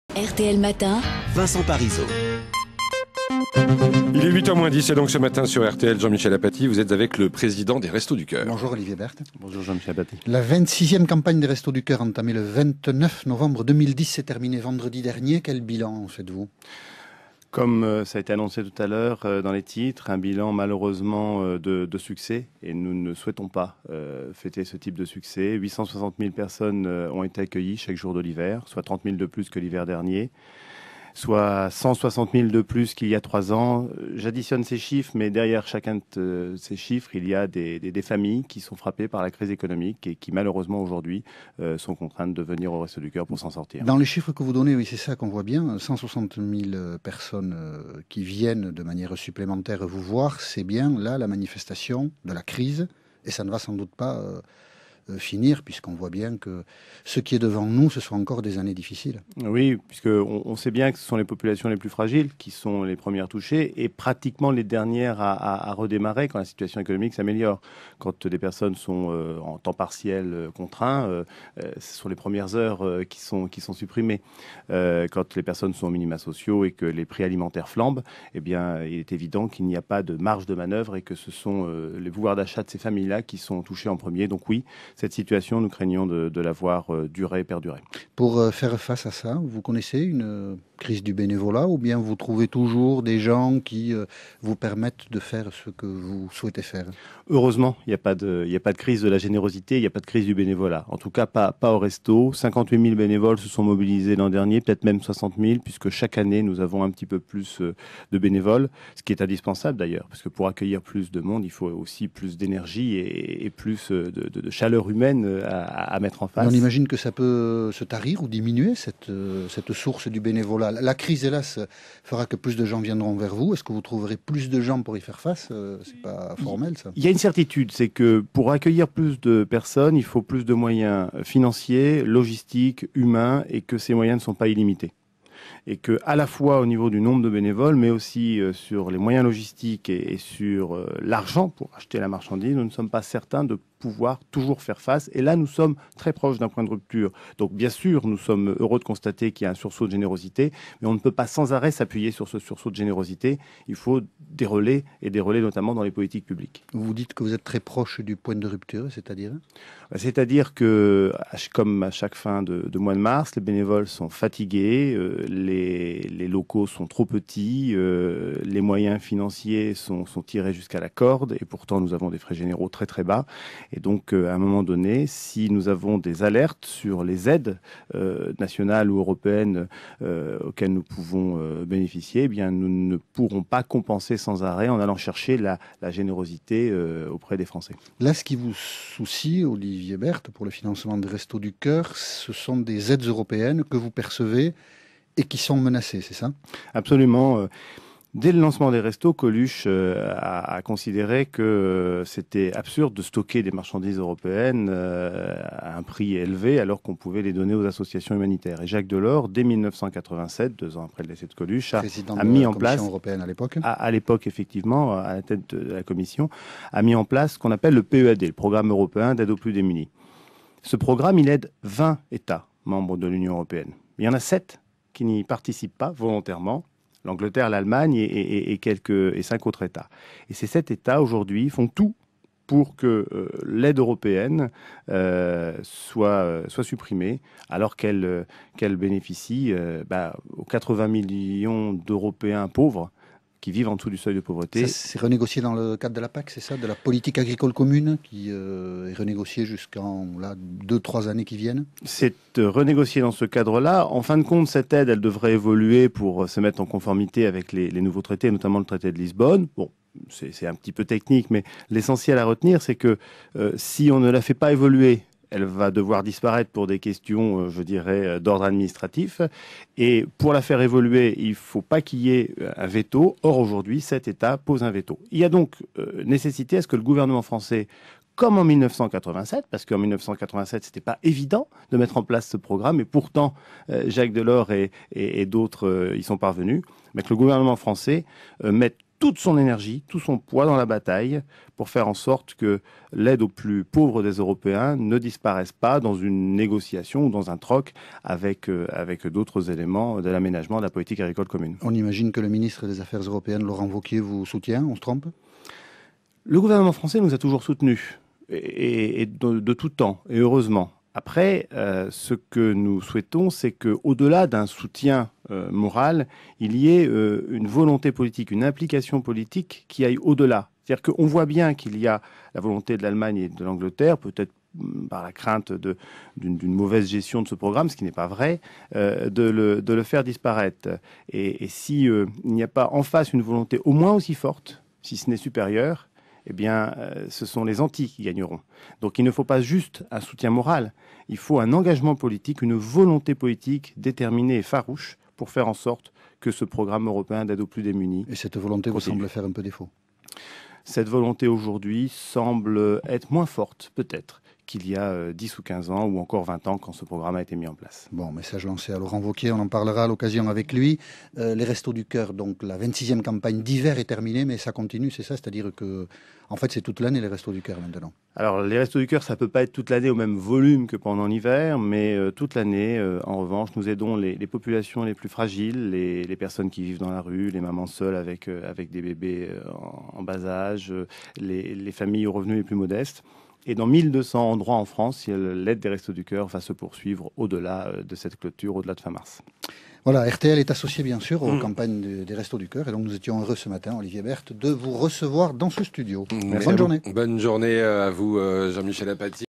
Olivier Berthe, président des Restos du Cœur : Il n'y a he - RTL - RTL
Olivier Berthe, président des Restos du Cœur : Il n'y a heureusement pas de crise du bénévolat aux Restos !